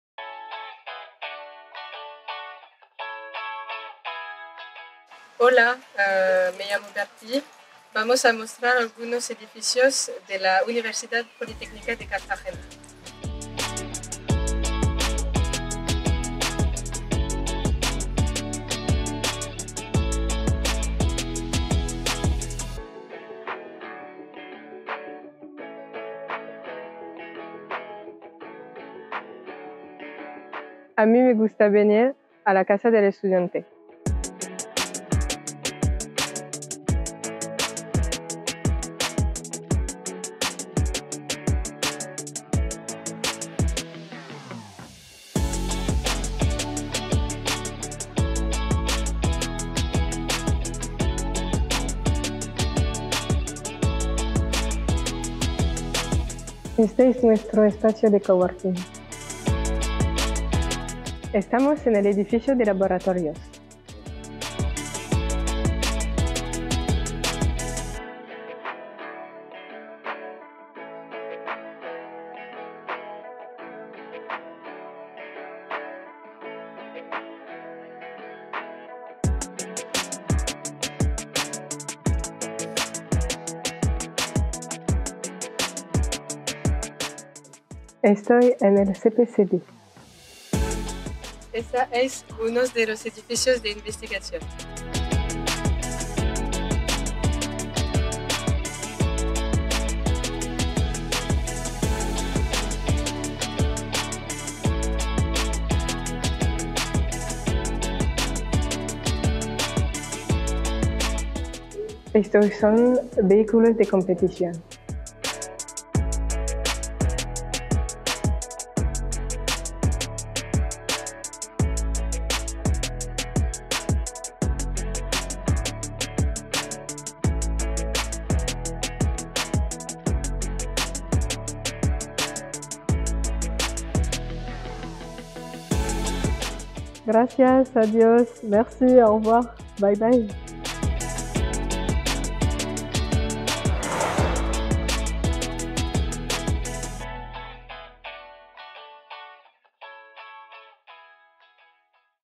Recorrido por el Campus Muralla del Mar
Bertille Defourny,  estudiante de Ingeniería de Telecomunicación de la Universidad Tecnológica de Troyes, del consorcio EUt+, muestra algunos de los espacios del Campus Muralla del Mar de la UPCT, donde realiza su estancia Erasmus este semestre. Vídeo realizado por Saad Hakki y Sarah Hniene, de UTT-EUt+, durante su Erasmus Staff.